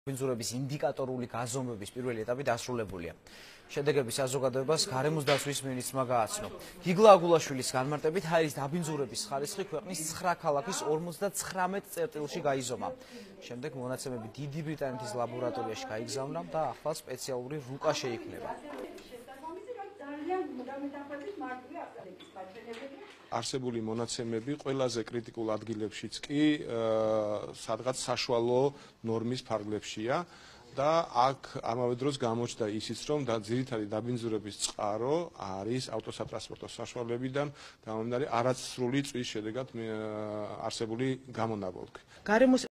გიგლა აგულაშვილი ატმოსფერული ჰაერის ინდიკატორული გაზომვების შედეგებზე
გარემოსა და ბუნებრივი რესურსების დაცვის მინისტრმა გიგლა აგულაშვილმა, გარემოს ეროვნული სააგენტოს უფროსთან ერთად ატმოსფერული ჰაერის ინდიკატორული გაზომვების შედეგები საზოგადოებას გააცნო.
გარემოს ეროვნულმა სააგენტომ მონიტორინგის გაძლიერებისა და ქსელის გაფართოების მიზნით ინდიკატორული გაზომვები განახორციელა. ატმოსფერული ჰაერის გაზომვები საქართველოს მასშტაბით ცხრა ქალაქის 58 წერტილში, მათ შორის თბილისში 26 წერტილში განხორციელდა.
ჰაერის დაბინძურების განმსაზღვრელი ინდიკატორული საზომები ორი კვირის განმავლობაში საქართველოს მასშტაბით სხვადასხვა წერტილში იყო განთავსებული და შემდეგ დიდი ბრიტანეთის აკრედიტებულ ლაბორატორიაში გაიგზავნა საანალიზოდ.